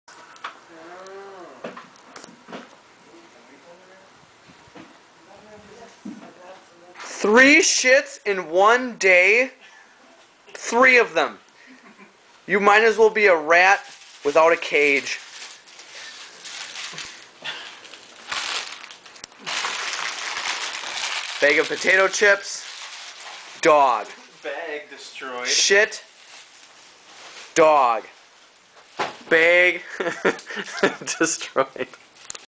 Berating Wrigley, version 2
So the dog crapped 3 times in the apartment in one 24-hour span.  Had to verbally punish said dog again.  Don't think this worked, as you see a tail wagging.

I've failed the Dumbing Down your Dog program.